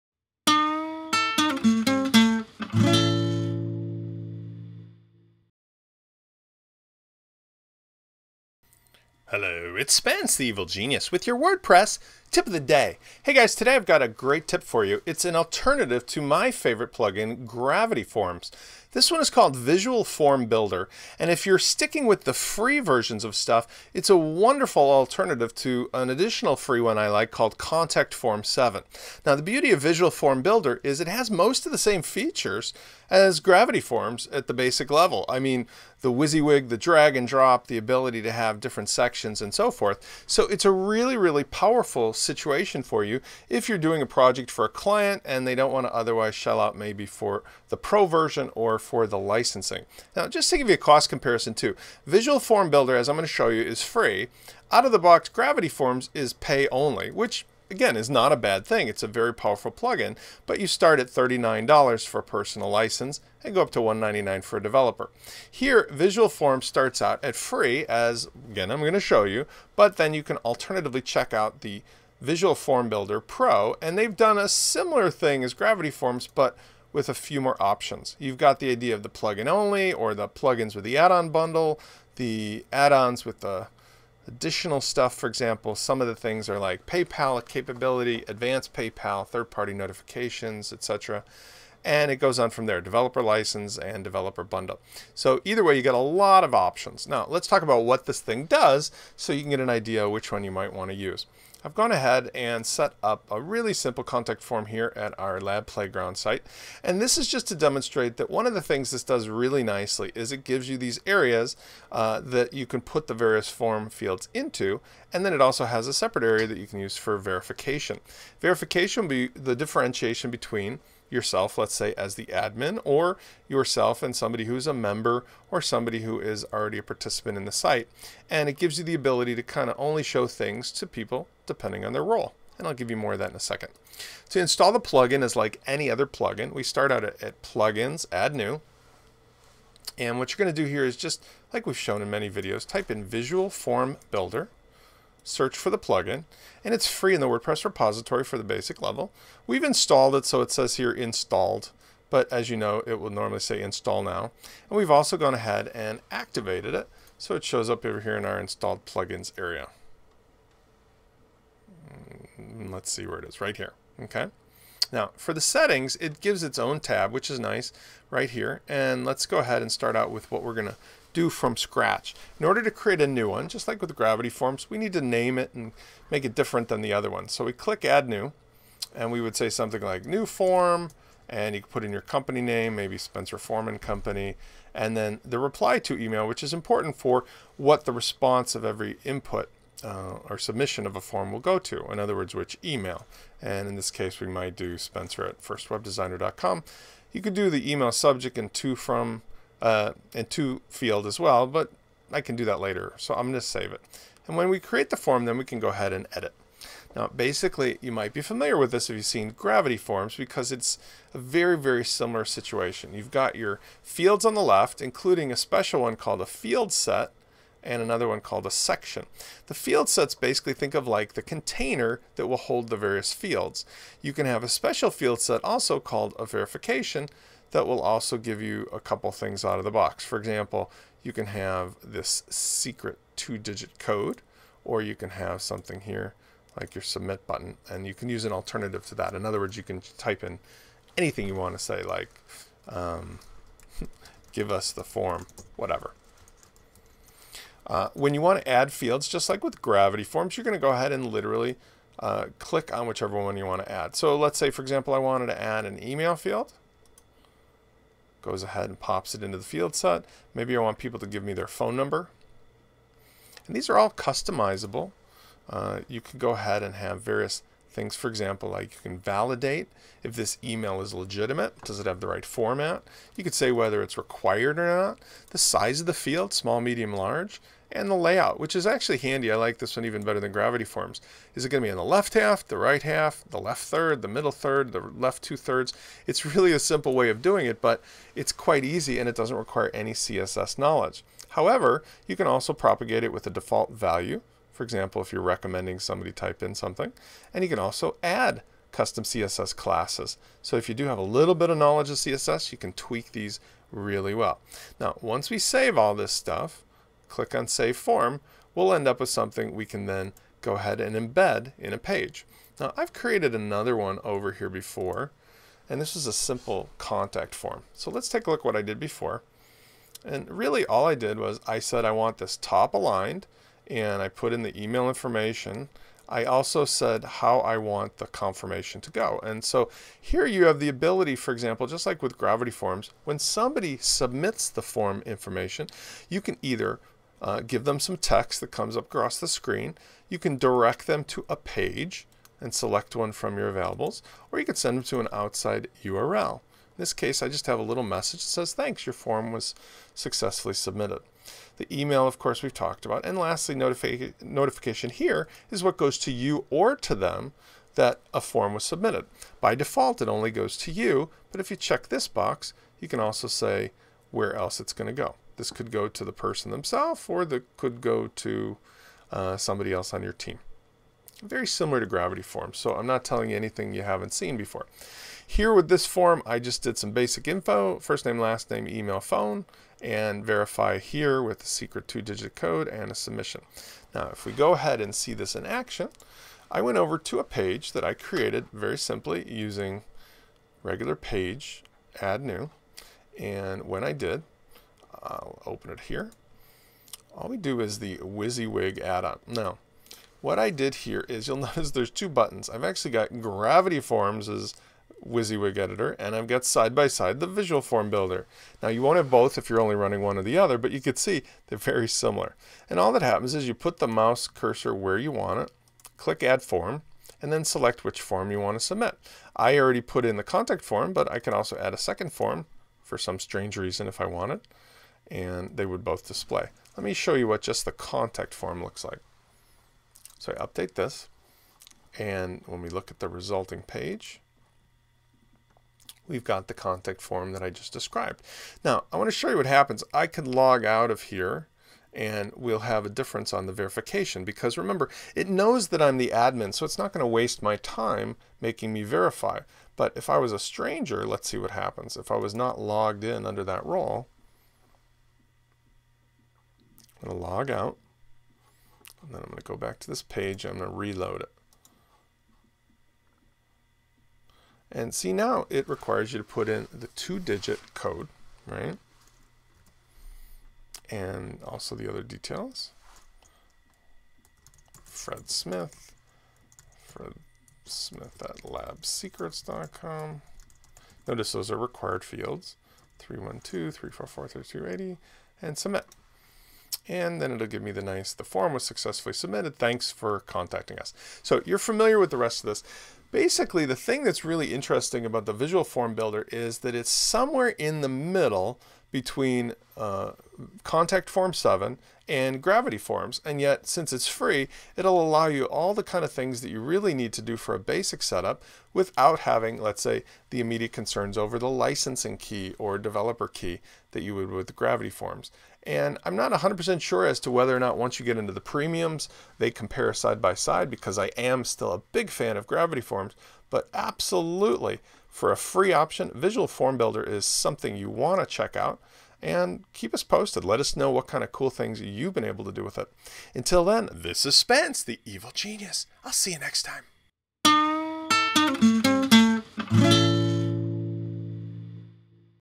A Free & Powerful Form Builder For WordPress
In this video, Spence shows you a wonderful alternative to using Gravity Forms or Contact Form 7 for building a client contact form on WordPress.

The plugin is called Visual Form Builder, and even in it's free version it provides most of the drag & drop features of Gravity Forms.

It is a wonderfully simply, yet powerful option between these two other popular plugins.